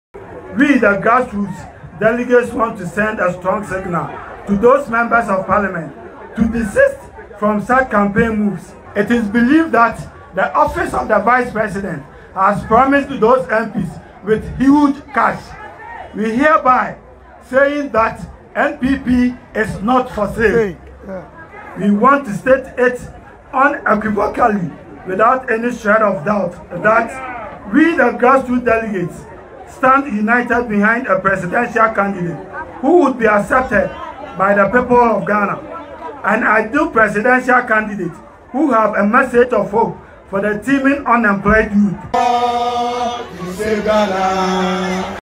Submissions of some polling station executives of NPP.
Some polling station executives of the ruling New Patriotic Party have threatened to organize and vote against any member of parliament who openly declare support for vice president Dr Mahamudu Bawumia before the party's presidential primaries.